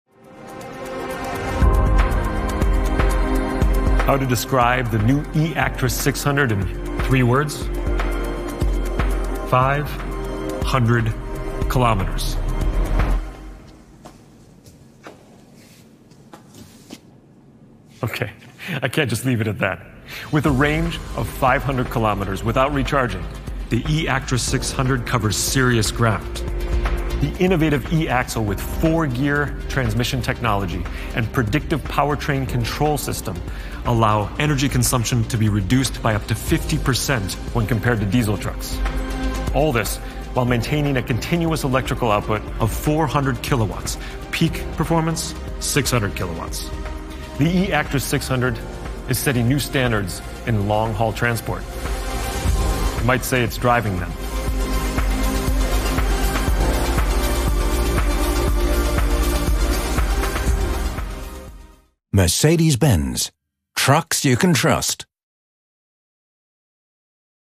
The new eActros 600: Goes further than you think | Mercedes-Benz Trucks
Long distance without charging worries: the eActros 600 can cover a range of 500 km without recharging.* And with its megawatt charging system, it can charge from 20% to 80% in around 30 minutes.** Its drive system creates extra power. What's more, the lithium iron phosphate (LFP) batteries don't require nickel or cobalt and offer huge advantages when it comes to both service life and energy flow.

*The range has been determined internally under specific test conditions, after preconditioning with a 4x2 semitrailer tractor with a gross vehicle combination weight of 40 t at an ambient temperature of 20°C in long-distance operation, and can deviate from the values determined in accordance with Regulation (EU) 2017/2400.
**Based on internal simulations because a binding and universal Megawatt Charging System (MCS) standard is currently still in development.